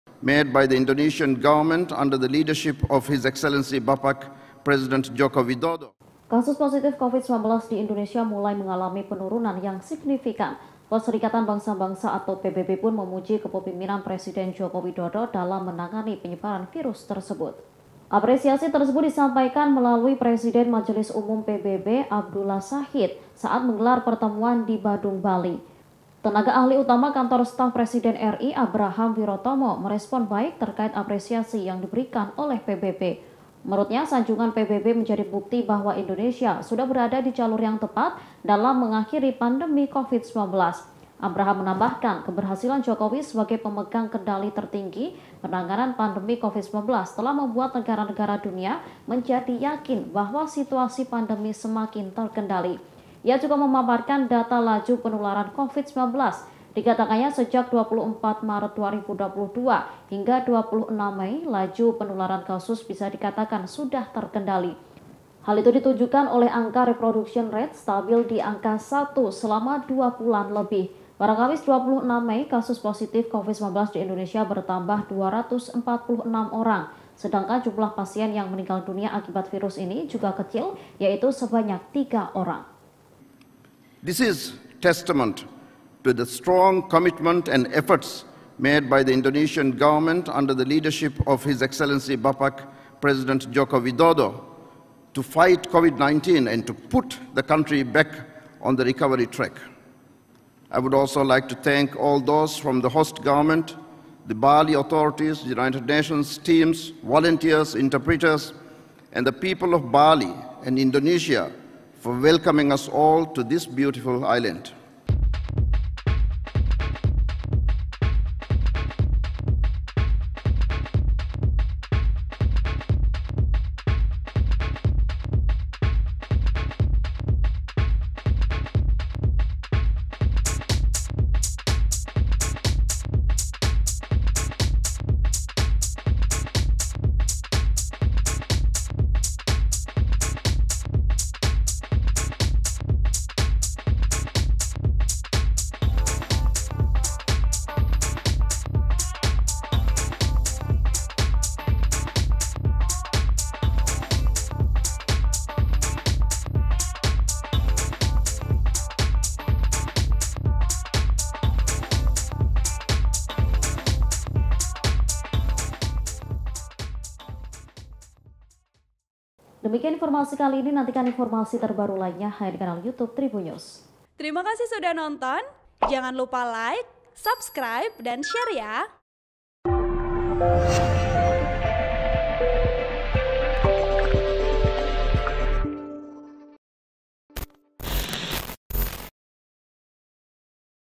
Jokowi Dipuji PBB karena Berhasil Kendalikan Laju Covid 19 di Indonesia, Bukti Komitmen Kuat
TRIBUN-VIDEO.COM - Kasus positif Covid-19 di Indonesia mulai mengalami penurunan yang signifikan.

Perserikatan Bangsa-Bangsa (PBB) pun memuji kepemimpinan Presiden Joko Widodo dalam menangani penyebaran virus tersebut.

Apresiasi tersebut disampaikan melalui Presiden Majelis Umum PBB, Abdulla Shahid, pada acara Global Platform for Disaster Risk Reduction (GPDRR) ke-7, di Badung, Bali.

Tenaga Ahli Utama Kantor Staf Presiden RI Abraham Wirotomo merespons baik terkait apresiasi yang diberikan PBB.

Menurutnya, sanjungan PBB menjadi bukti bahwa Indonesia sudah berada di jalur yang tepat dalam mengakhiri pandemi Covid-19.

Abraham menambahkan, keberhasilan Jokowi sebagai pemegang kendali tertinggi penanganan pandemi Covid-19 telah membuat negara-negara dunia menjadi yakin bahwa situasi pandemi semakin terkendali.

“Tidak mungkin tujuh ribu delegasi dari 185 negara berani datang ke Indonesia jika tidak yakin Indonesia berhasil mengendalikan Covid-19,” kata Abrahan kepada wartawan, Jumat (27/5/2022)

Ia juga memaparkan data laju penularan Covid-19.

Dikatakannya, sejak (24/3/2022) hingga (26/5), laju penularan kasus bisa dikatakan sudah terkendali.

Hal itu ditunjukkan oleh angka Reproduction Rate stabil di angka 1 selama dua bulan lebih.

Pada Kamis (26/5), kasus positif Covid-19 di Indonesia bertambah 246 orang.

Sedangkan jumlah pasien yang meninggal dunia akibat virus ini juga kecil, yaitu sebanyak tiga orang.

(Tribun-Video.com/Tribunnews.com)

Artikel ini telah tayang di Tribunnews.com dengan judul PBB Puji Indonesia Soal Penanganan Pandemi, KSP: Kita di Jalur yang Tepat,

Penulis: Reza Deni
Editor: Malvyandie Haryadi